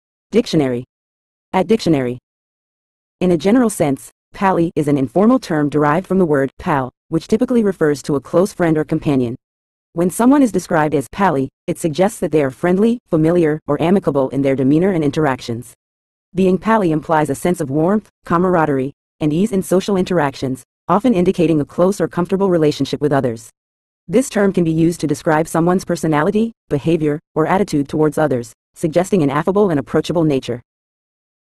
Pally Meaning In English
In a general sense, "pally" is an informal term derived from the word "pal," which typically refers to a close friend or companion. When someone is described as "pally," it suggests that they are friendly, familiar, or amicable in their demeanor and interactions. Being pally implies a sense of warmth, camaraderie, and ease in social interactions, often indicating a close or comfortable relationship with others. This term can be used to describe someone's personality, behavior, or attitude towards others, suggesting an affable and approachable nature.